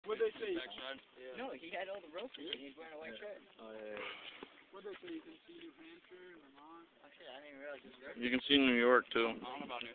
Poke-O-Moonshine Mountain
This is from a 1.2 mile hike to the top of Poke-O-Moonshine Mountain which is located in Keeseville, NY.